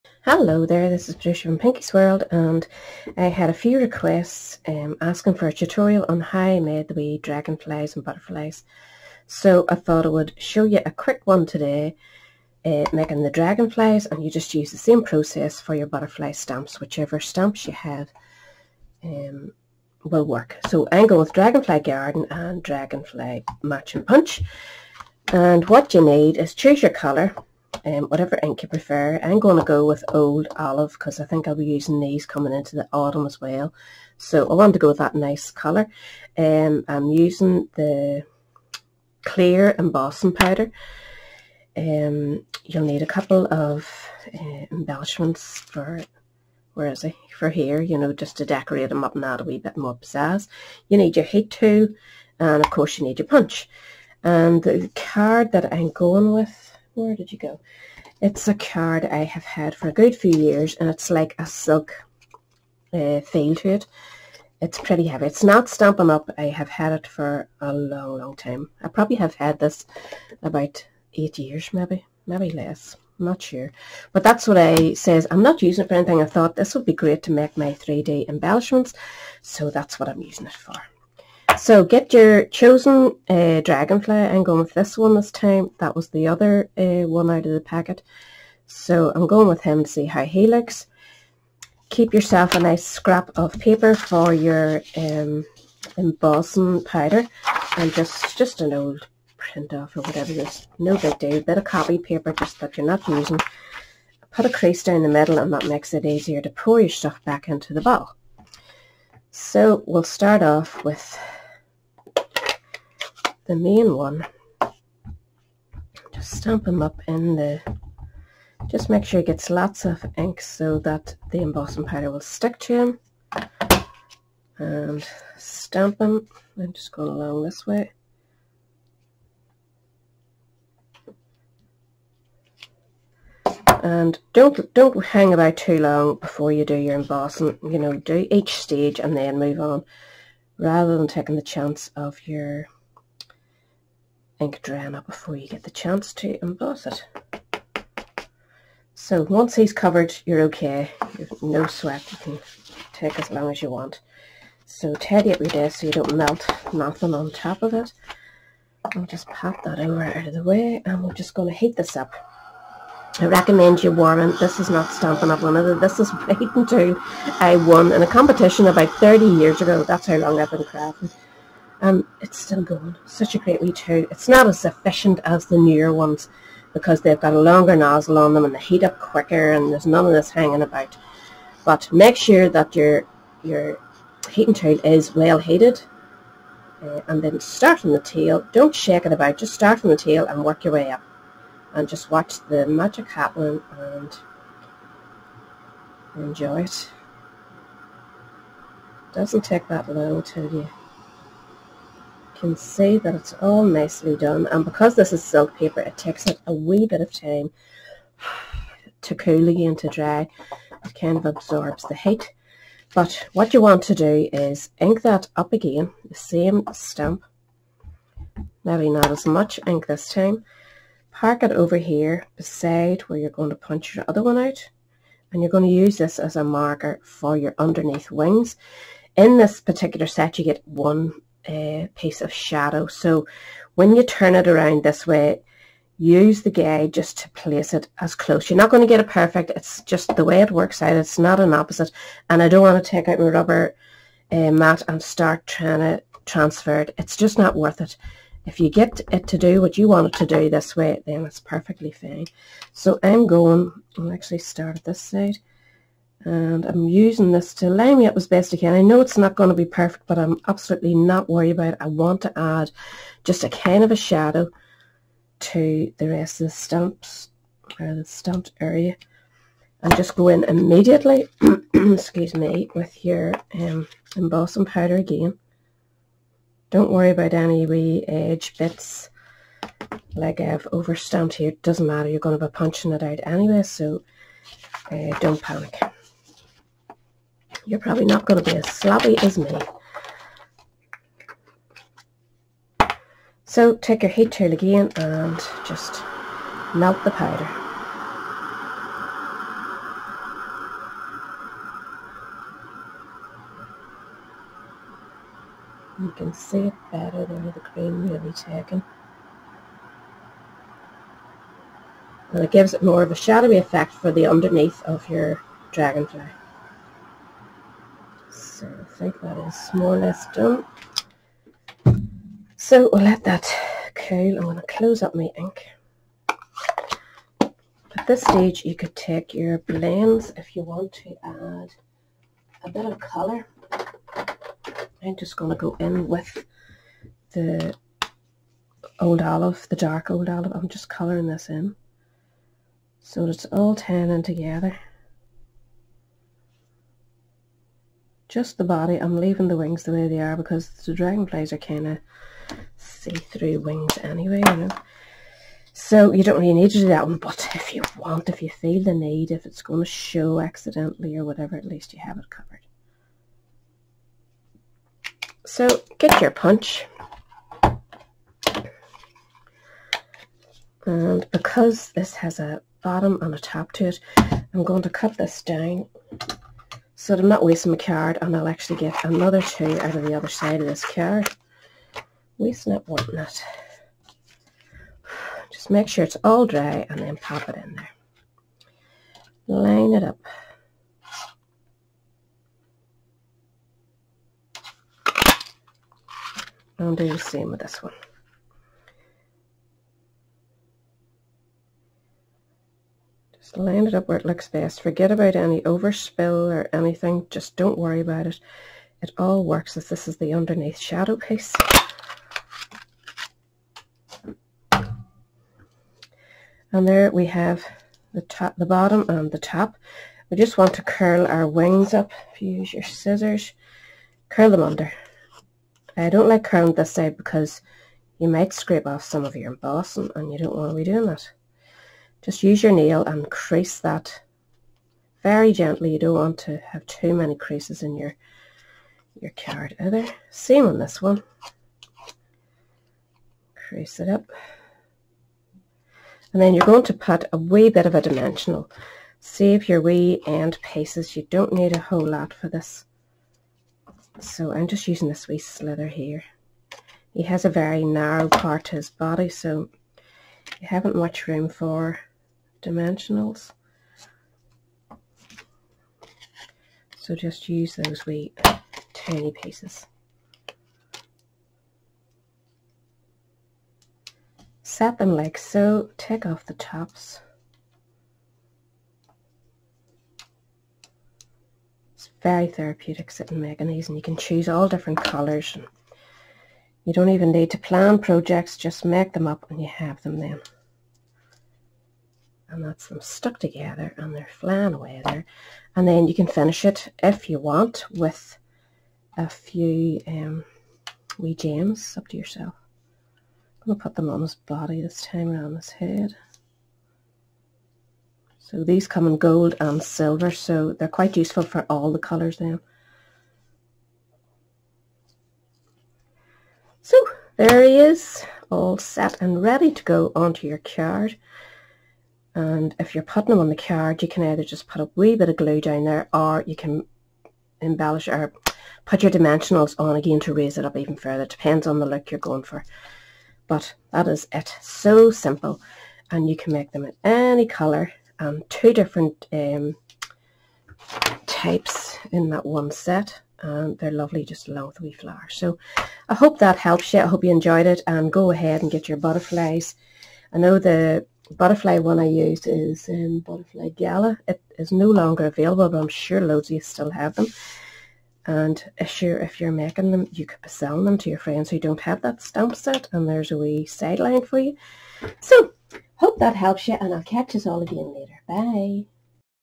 How to create 3d dragonfly embellishments
Pinky O'Hagan gives step by step instruction showing how to make this fun project.